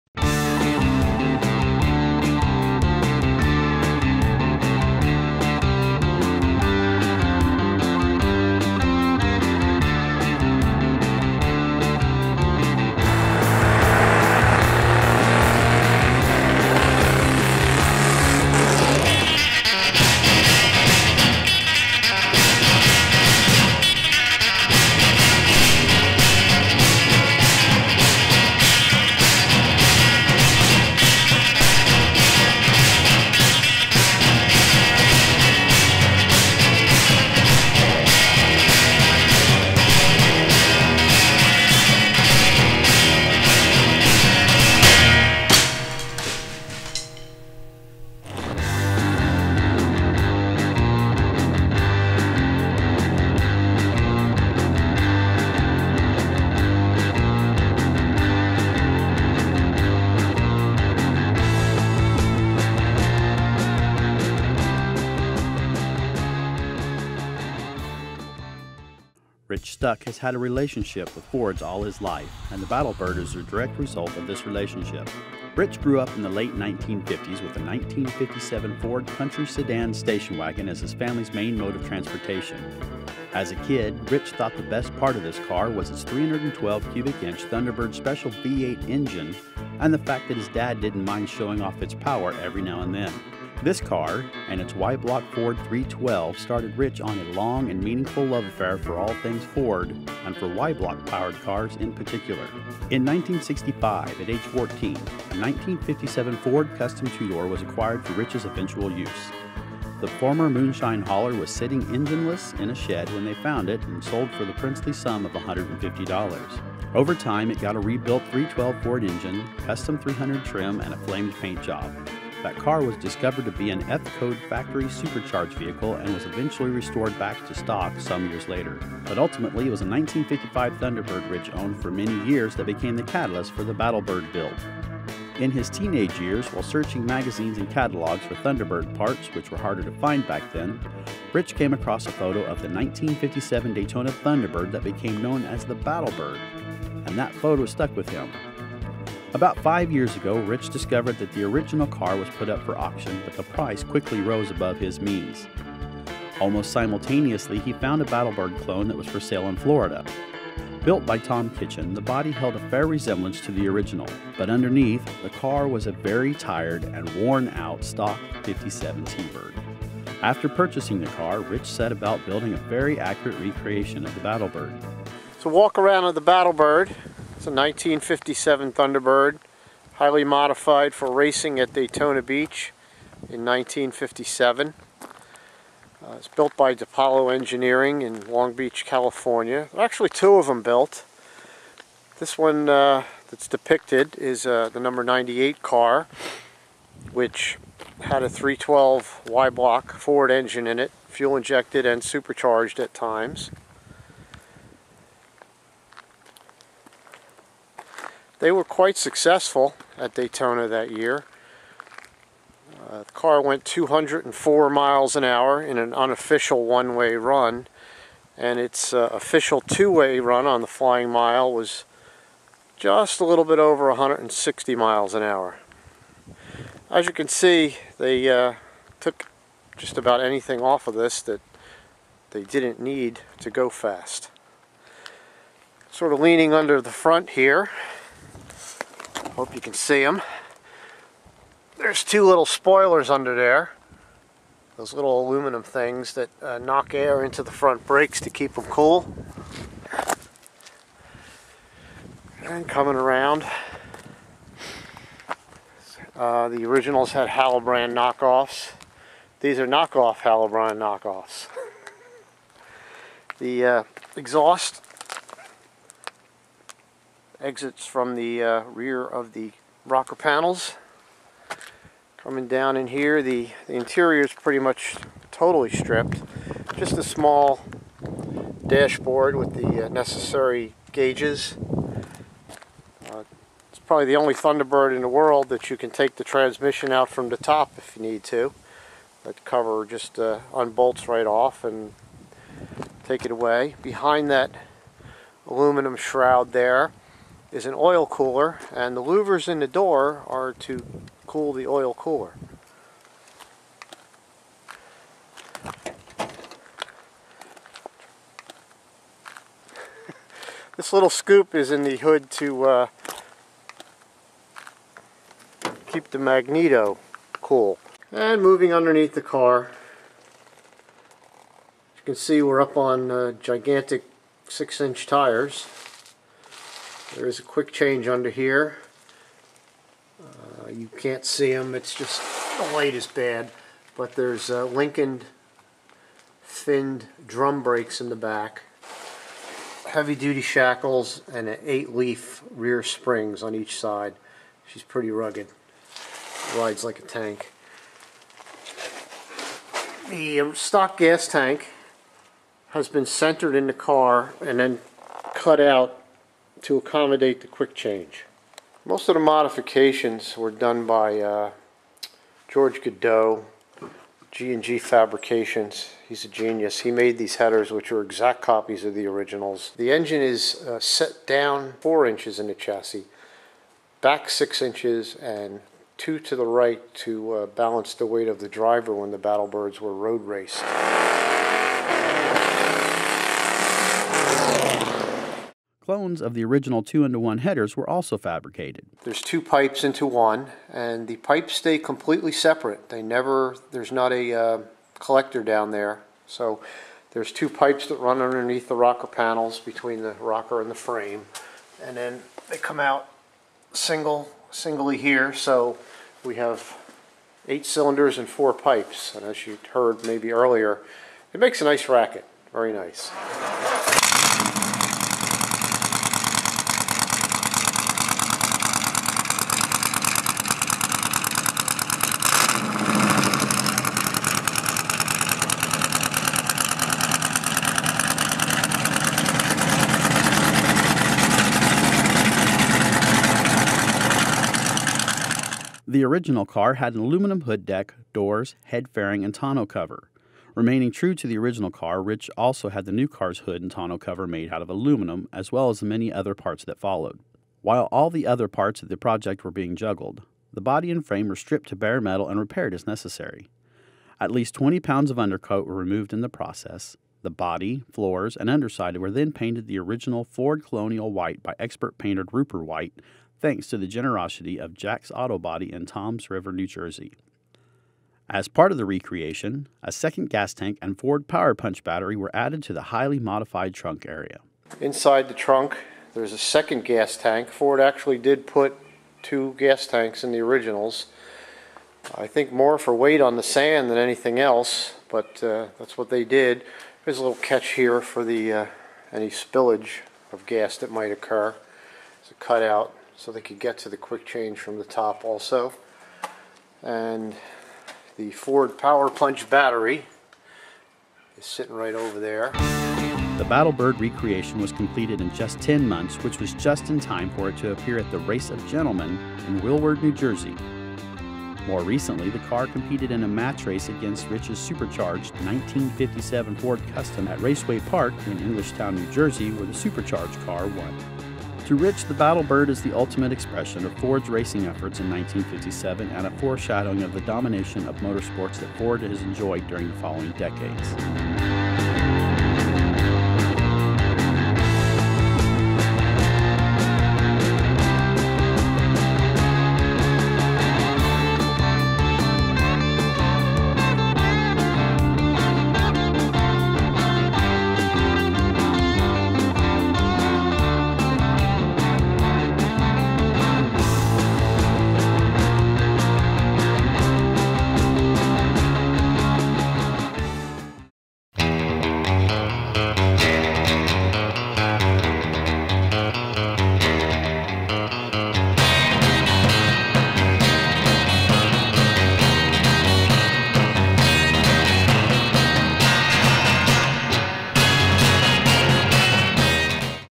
1957 Ford Thunderbird "Battlebird" - Speed & Chrome Illustrated Magazine
Rich Stuck has had a relationship with Fords all his life and the Battlebird is a direct result of this relationship. Rich grew up in the late 1950s with a 1957 Ford country sedan station wagon as his family's main mode of transportation as a kid Rich thought the best part of this car was it's 312 cubic inch Thunderbird special V8 engine, and the fact that his dad didn't mind showing off it's power every now and then. This car and its Y-block Ford three-twelve started Rich on a long and meaningful love affair for all things Ford and for Y-block powered cars in particular. In 1965 at age 14,  a 1957 Ford custom two door was acquired for Rich's eventual use. The former moonshine holler was sitting engineless in a shed when they found it and sold for the princely sum of one hundred and fifty dollars. Overtime it got a rebuilt three-twelve Ford engine custom 300 trim and a flamed paint job. That car was discovered to be a f-code factory supercharge vehicle and was eventually restored back to stock some years later, but ultimately it was a 1955 Thunderbird Rich had owned many years that became the catalyst for the Battlebird build. In his teenage years while searching magazines and catalogs for Thunderbird parts which were harder to find back then, Rich came across a photo of a 1957 Daytona Thunderbird that became known as the Battlebird, and that photo stuck with him. About five years ago Rich discovered that the original car was put up for auction but the price quickly rose above his means. Almost simultaneously he found a Battlebird clone that was for sale in Florida. Built by Tom Kitchen, the body held a fair resemblance to the original, but underneath the car was a very tired and worn out stock '57 T-bird. After purchasing the car Rich set about building a very accurate recreation of the Battlebird.